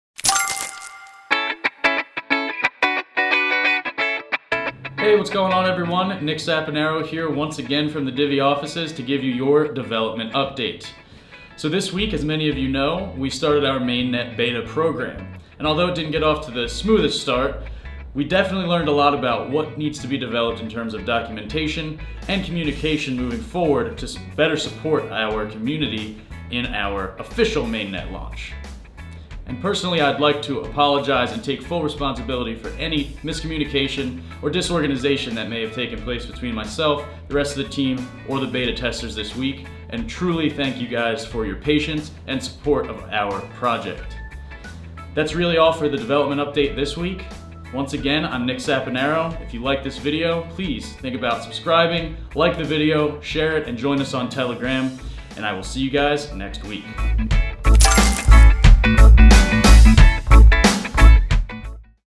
Divi Project Weekly Update May 18th 2018 - Beta Testing Program
The Divi team brings us our weekly development update.

Check out the video & click the link below to sign up for our soon to release Beta.

Divi - Crypto - Blockchain - Masternodes